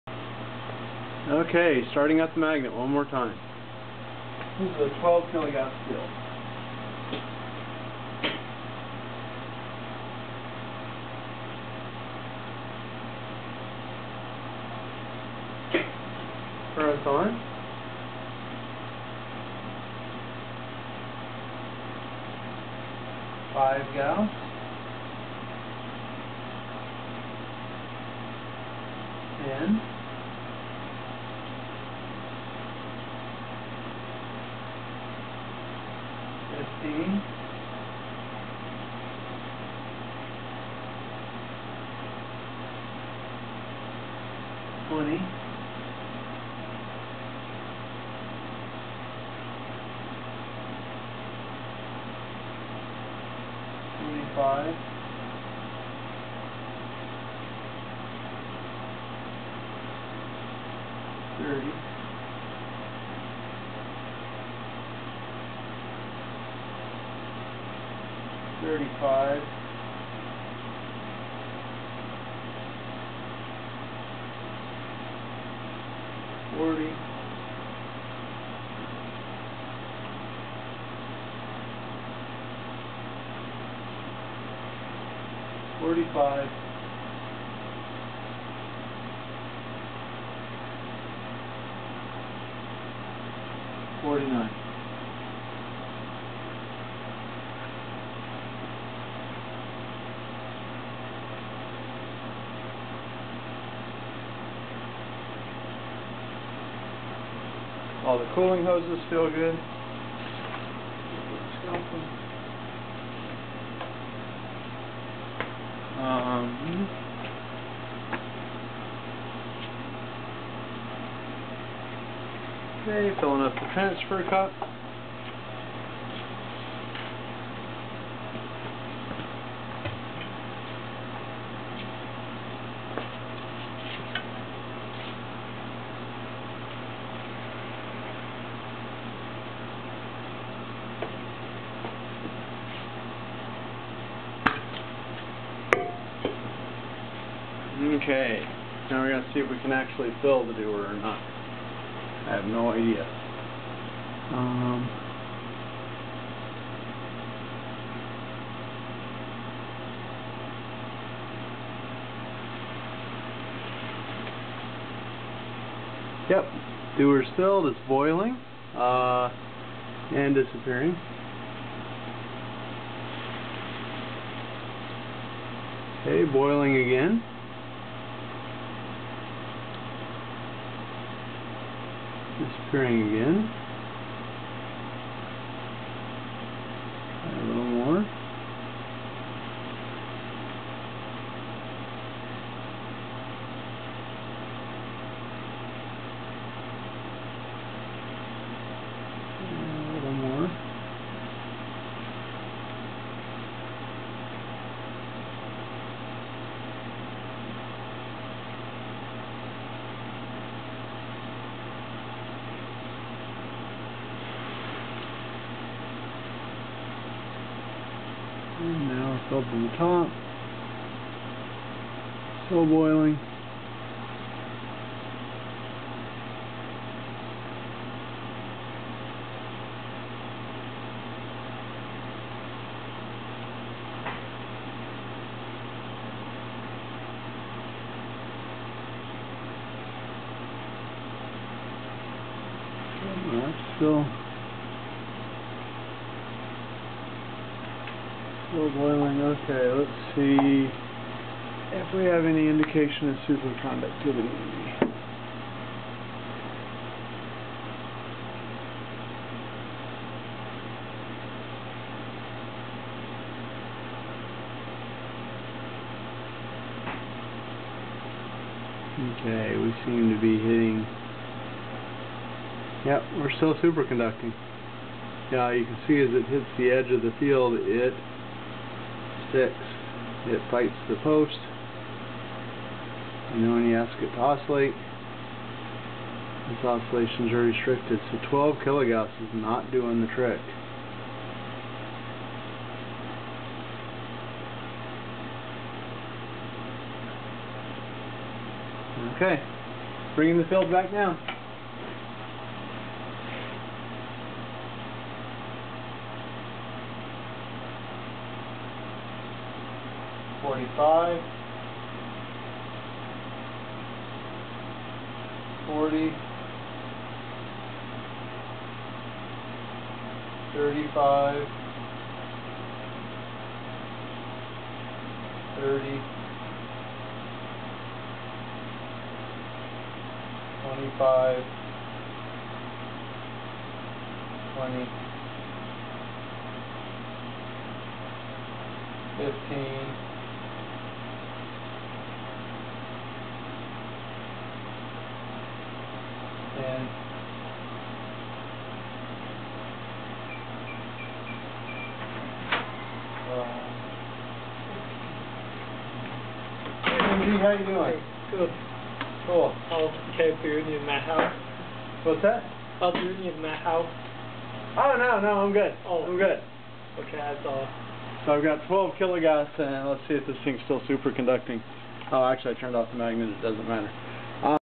Testing for YBCO Supeconductor Quench via Magnetic Force
A YBCO superconductor is placed between the poles of a very uniform magnet and then cooled into its superconducting state.  The field frozen into the sample at the state transition opposes the fringing fields on the magnet.  However, had the magnetic field been strong enough to quench the superconductor, the results would have been the pendulum swinging freely beyond the pole pieces' diameter.